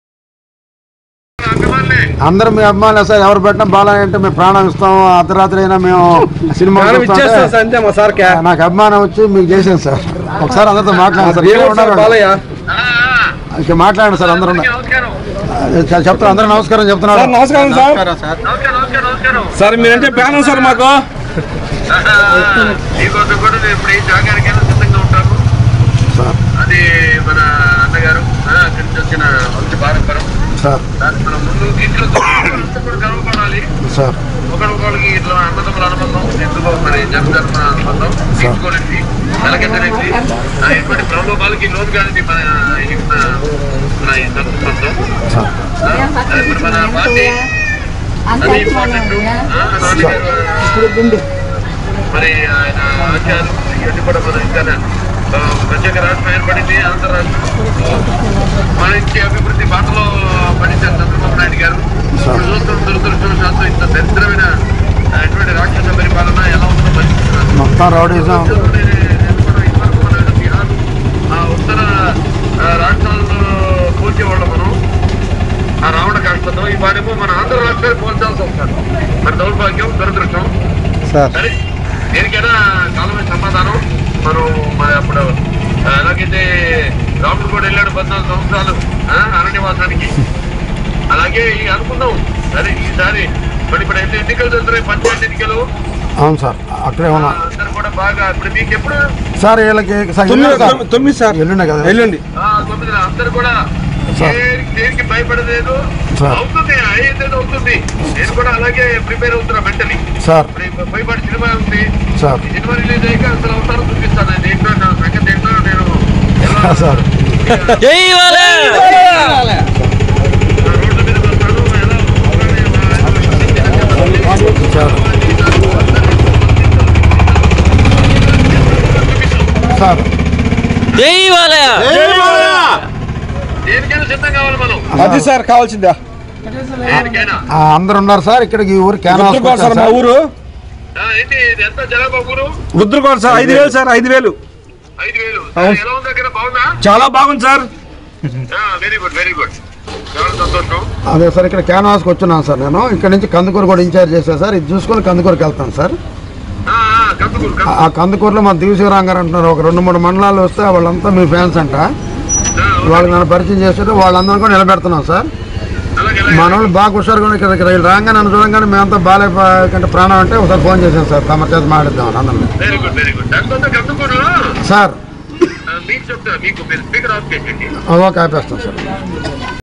Nandamuri Balakrishna Phone Call To Nellore TDP Leaders | Distoday News
TDP MLA Nandamuri Balakrishna Phone Call To Nellore TDP Leaders.Distoday News